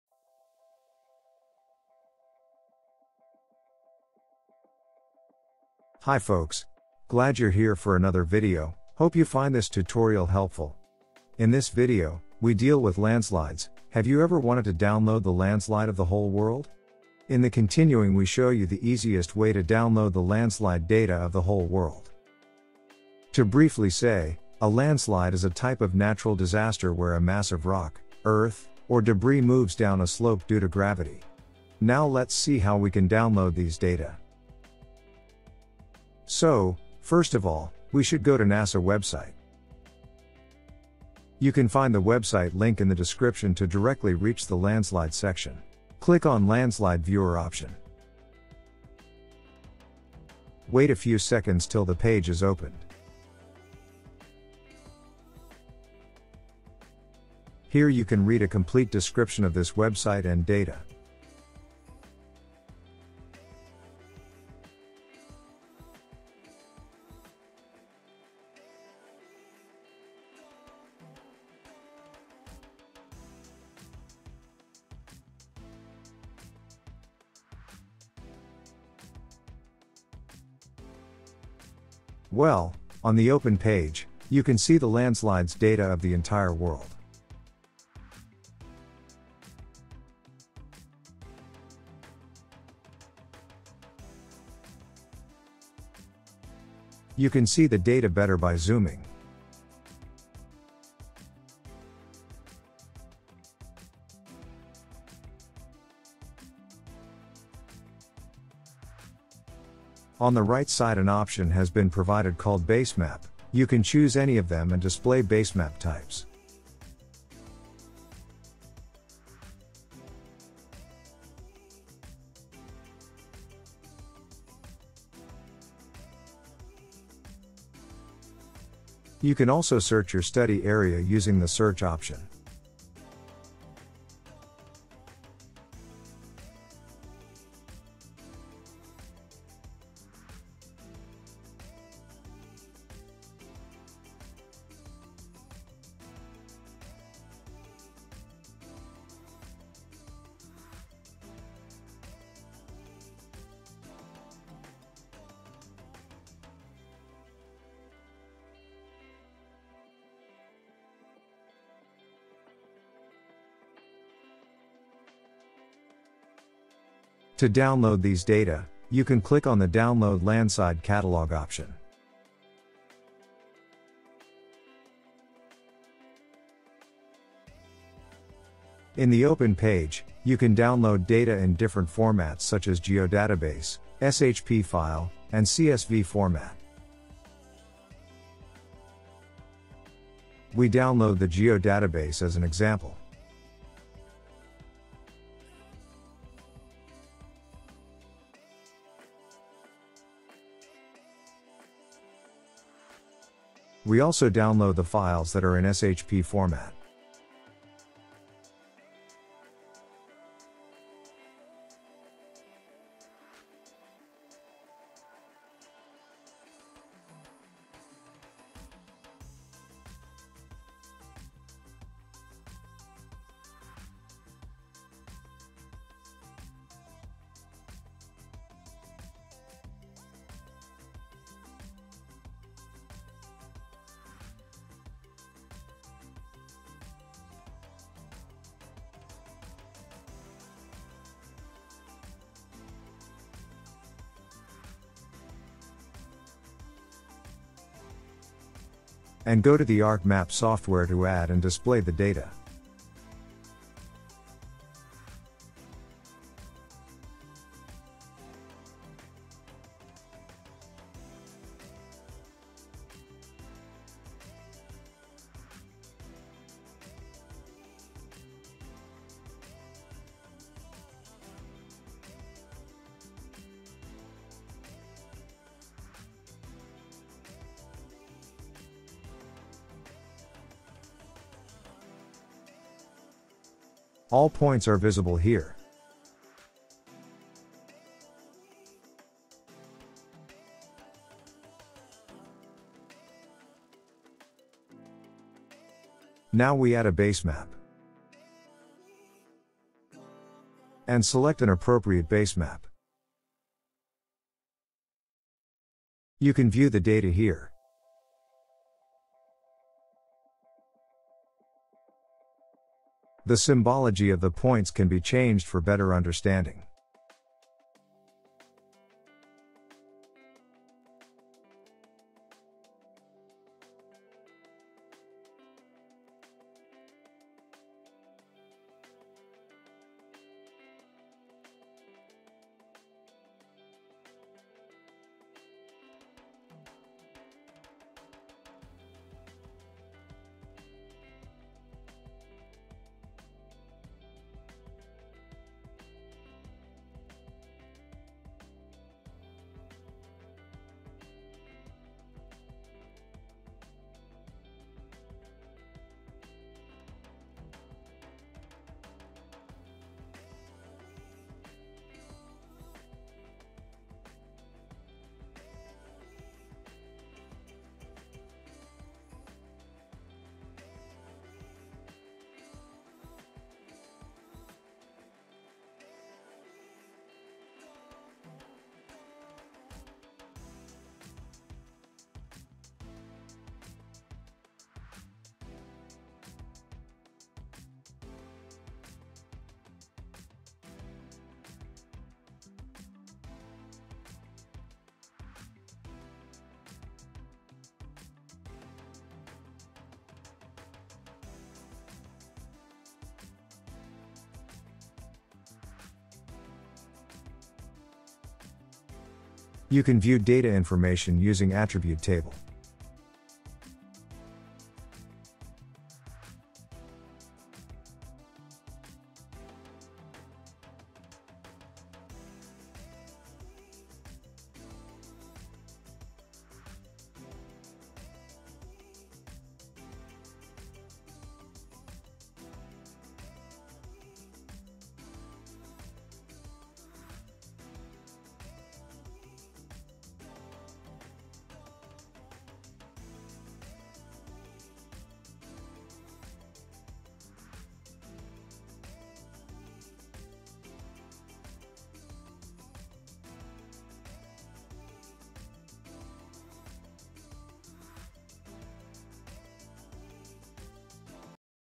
Download Landslide data for the whole world - Inventory map from NASA
A landslide is a type of natural disaster where a mass of rock, earth, or debris moves down a slope due to gravity. A landslide inventory is a detailed register of the spatial distribution, geometry, and attributes of landslides and is essential for landslide hazard analysis, risk management, regional planning, and land use management and development, especially in landslide-prone regions. NASA is building the biggest open global landslide inventory with the help of citizen scientists.
in this tutorial, we download the Landslide data from the NASA website and display it in the ArcGIS (Arc Map) software, we hope this is informative for you.